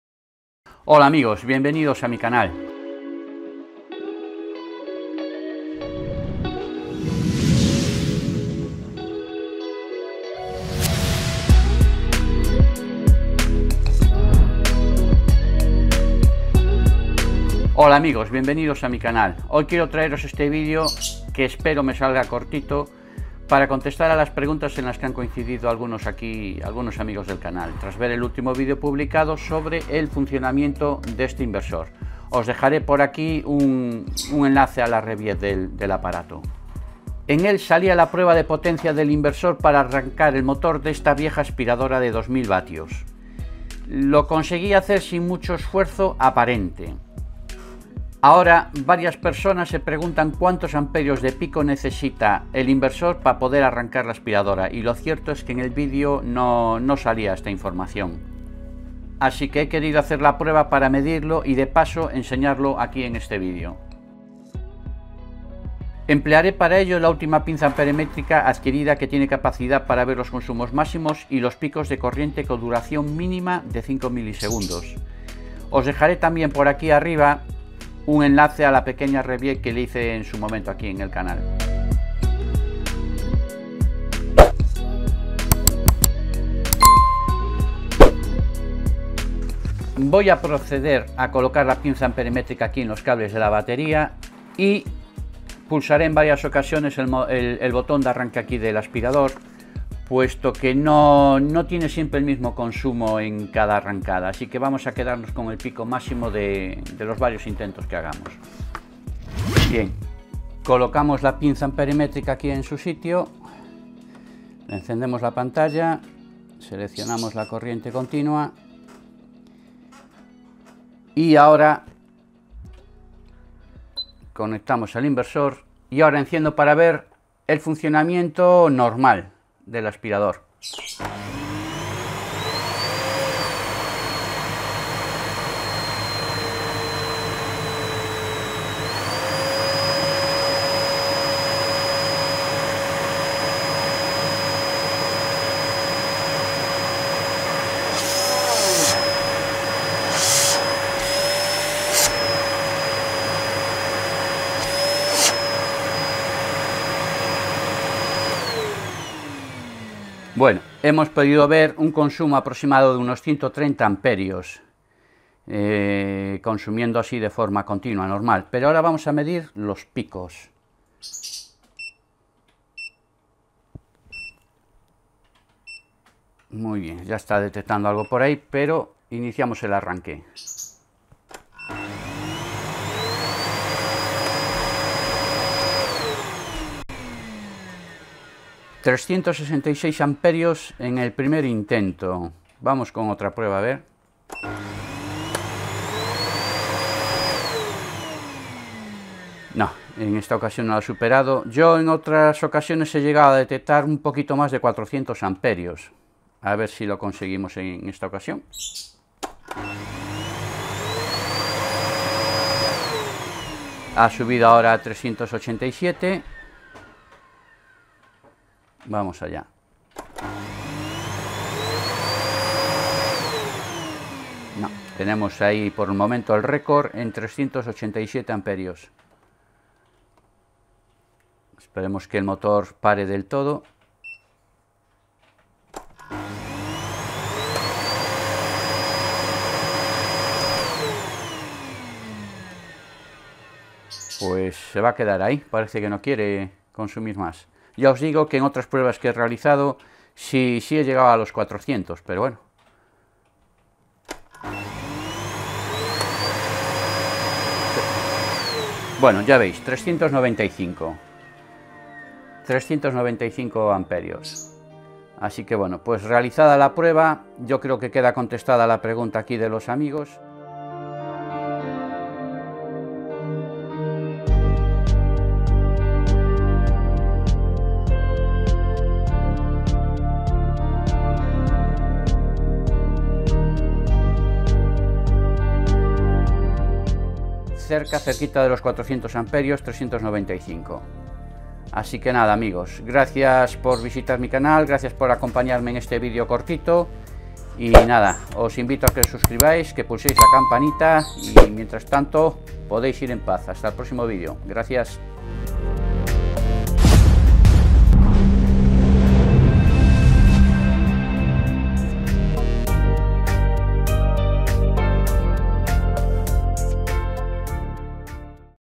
CONSUMO EN ARRANQUE DE MOTOR ELÉCTRICO CON INVERSOR (PICO MÁXIMO DE CORRIENTE).
COMPROBAMOS EL PICO DE CONSUMO ELECTRICO DE UN MOTOR DE UN ASPIRADOR CONECTADO A UN INVERSOR DE CORRIENTE.